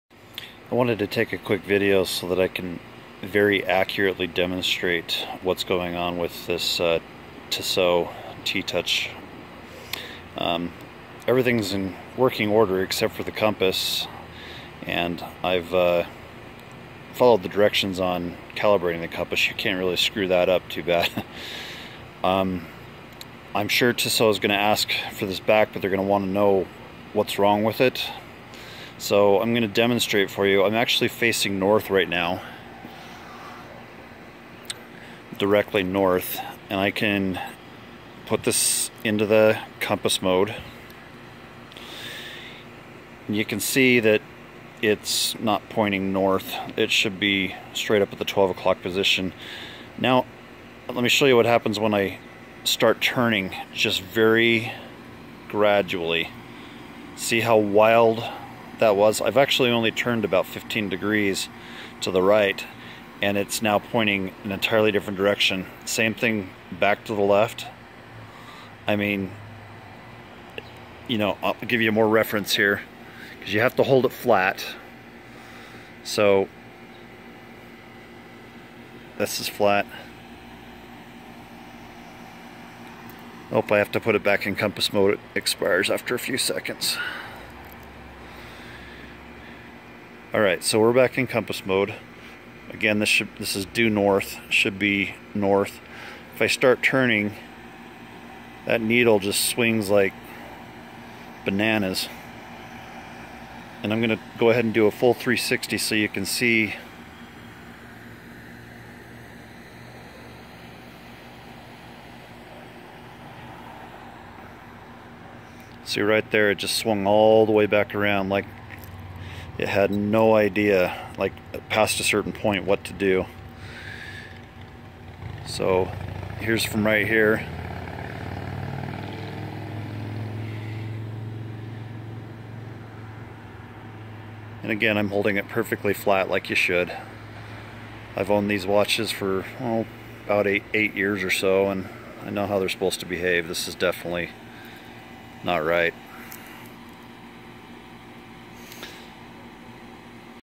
Fix/Solved: Tissot T-Touch Expert Solar Compass Malfunction (how-to re-calibrate in the description)
Tissot T-Touch Expert Solar Compass Malfunction and how to recalibrate.  Read Below for the procedure.

The video only shows how my Tissot was super confused in Compass mode.  This procedure BELOW is different than the CAL procedure you can get to by pressing T, Compass, Compass, Compass.  My feeling is that if your Tissot was doing what mine was and wildly changing directions, then this procedure is for you.

Update 2016/11/30 User Paulo Serra has indicated that he solved this problem by going into Service Mode and re-calibrating the compass.  Do this at your own risk.  I won't be held responsible if you really mess things up!  :)

To Enter Sleep Mode:
Press T
Touch Center Crystal to OPT
Press T
Touch Center Crystal to ECO
Hold + for two seconds
Press + to set the display to "Yes"
Press T and in 5 seconds it will go to power save mode

To Enter Service Mode:
With the watch in power save mode you are now ready to enter Service mode.
Press + for 3 seconds (nothing will happen)
Touch the 6 o'clock position
Press + 8 times to get to CAL

TO CALIBRATE COMPASS
Now find North and position yourself and your Tissot pointing due North.

The screen should be reading CAL with a little "?" up in the right corner.

With you and your Tissot at the 12 o'clock position both facing True North do the following:

Touch Center Crystal (the numbers will start to flash and change and the hands will still be at the 12 o'clock position)

While holding the 12 o'clock position due north, tap the center of the crystal again.

The hands will then move to the 11 o'clock position.  Rotate your Tissot a little counter-clockwise so that the hands are again pointing North.

Touch the Center Crystal (the hands will now move to the 10 o'clock position)

Rotate the watch counter-clockwise so that the hands are once again pointing due North.

Repeat this for every hour (9, 8, 7, etc.) face until you get to the end.  Hopefully you will see CAL and OK on the screen.  If you don't, repeat the procedure.

Now that you have set all of this up using True North, go ahead and set your Magnetic Declination for your area of the world and you're good to go.

If this doesn't work, you probably have bigger problems and should seek repair through an Authorized Tissot Service Center

Good luck.